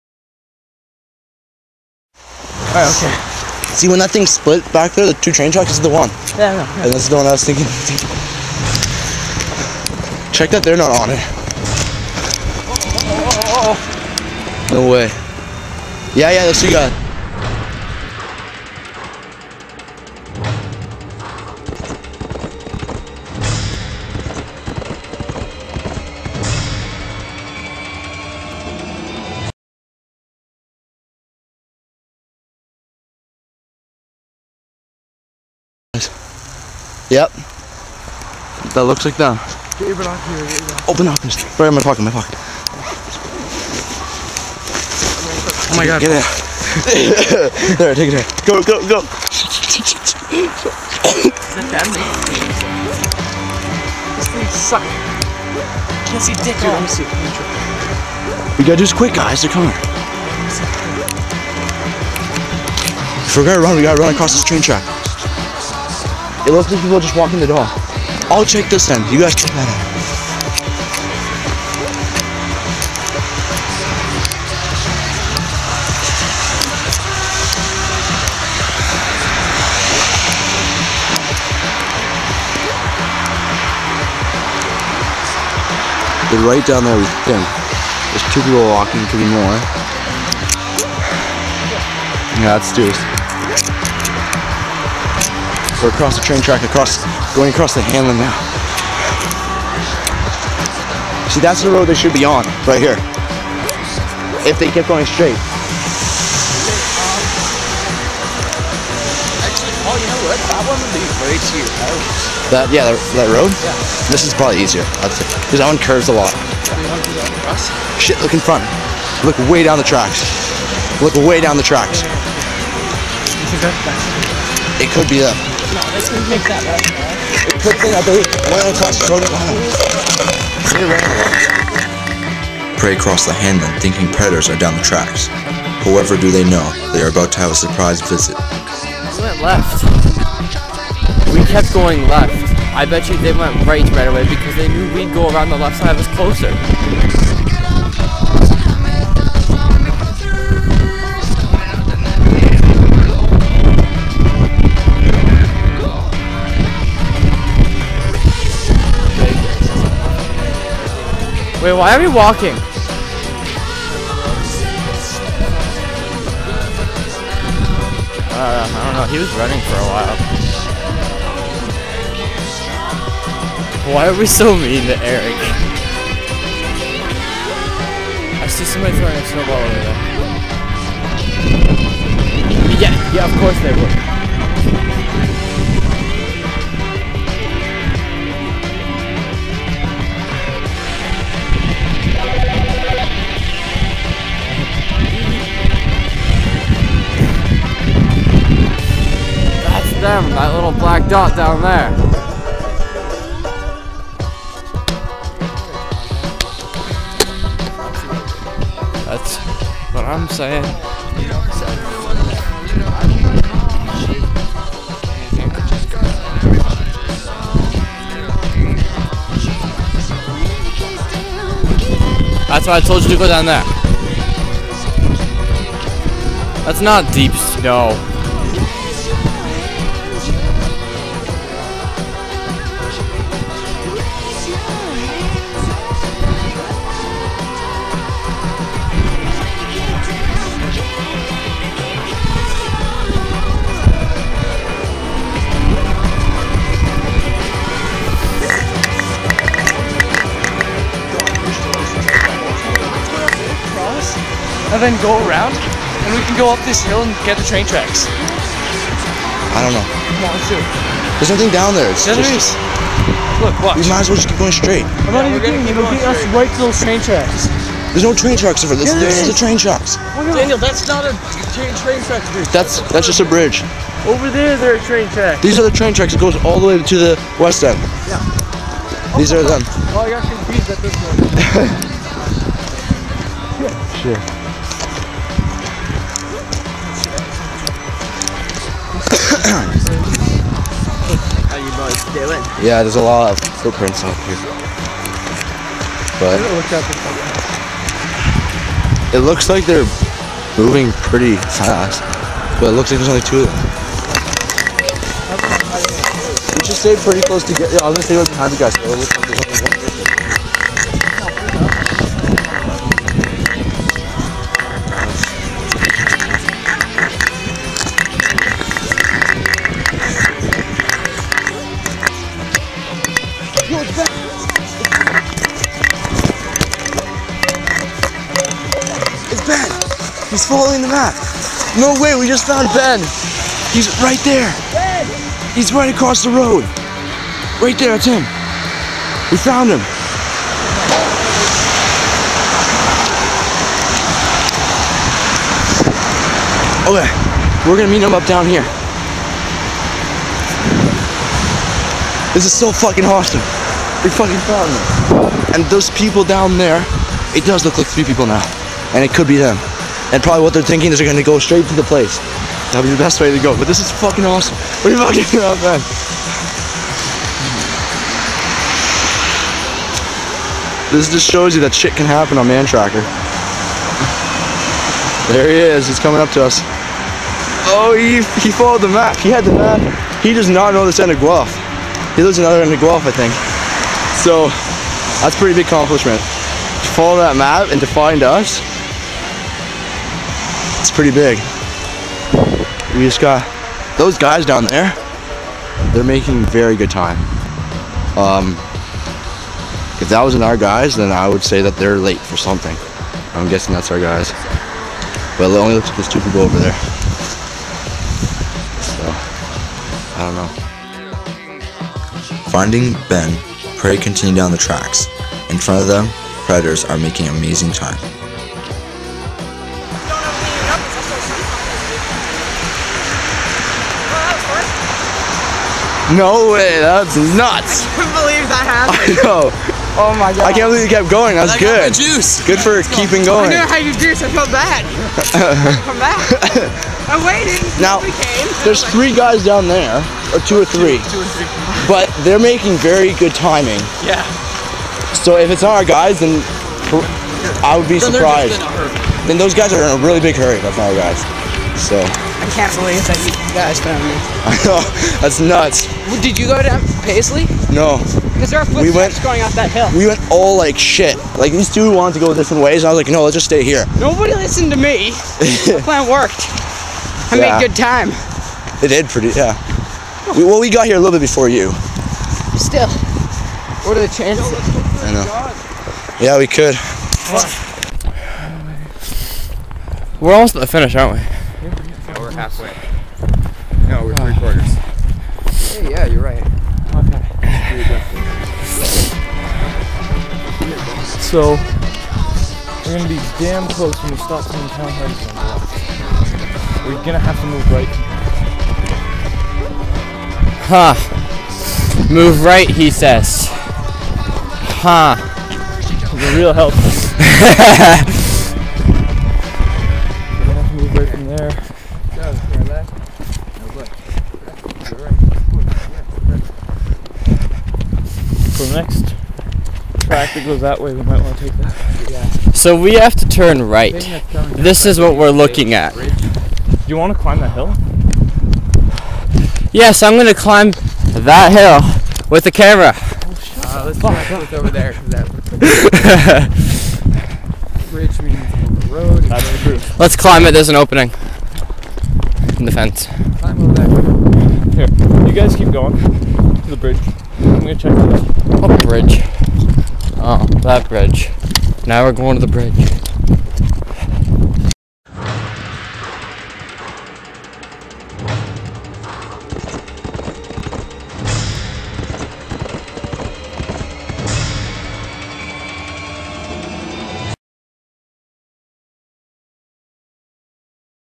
Guelph Mantracker Episode 1 (Pt. 4)
A Mantracker-like event across Guelph. 3 Predator, 4 Prey, who will survive?
Part 4
(Reality)

FlipArtists 2011

Credit:
Paul Olszewski
Ben Jackson
Eric Curwen
Tom Wall
Ian Ferris
Jeremy Eddy
Daniel Cigolea